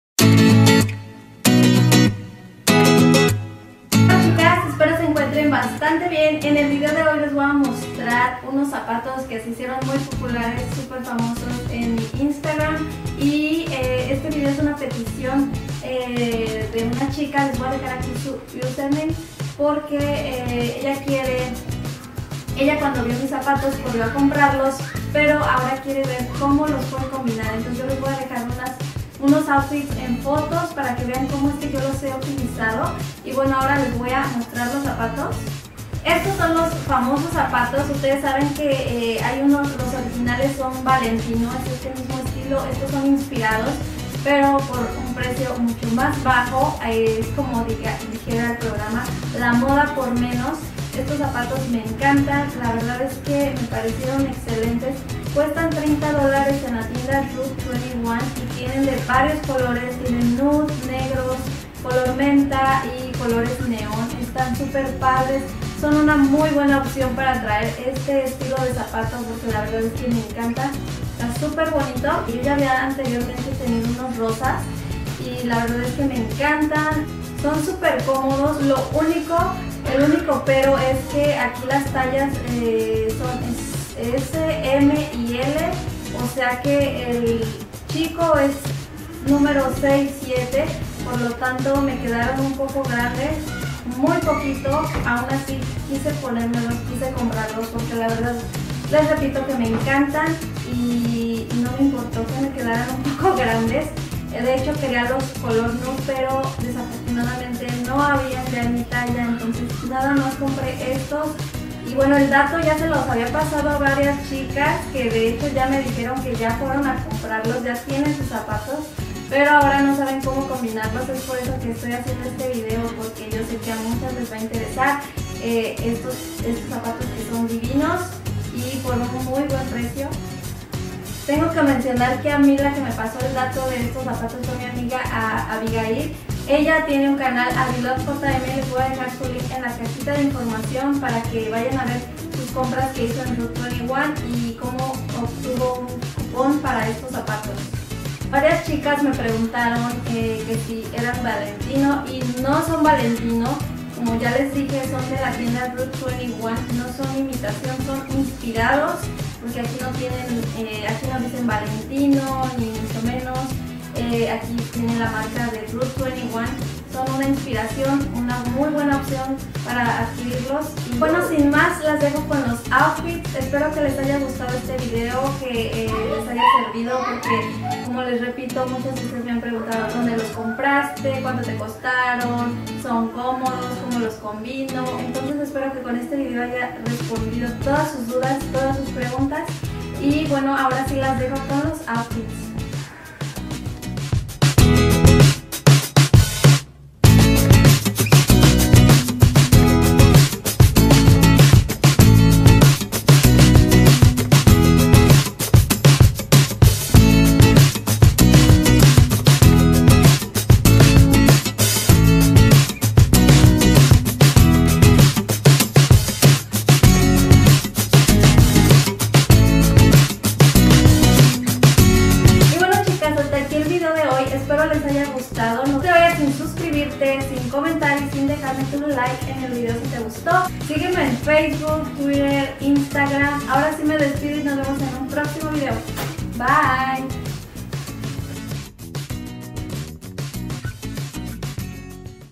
La MODA por MENOS // TACONES inspirados en los VALENTINO
El video de hoy es una peticion, espero les guste y sobre todo les sirva la info de los zapatos, porque siempre hay opciones, la clase y el estilo esta en uno, no importa las marcas...

SIGUEME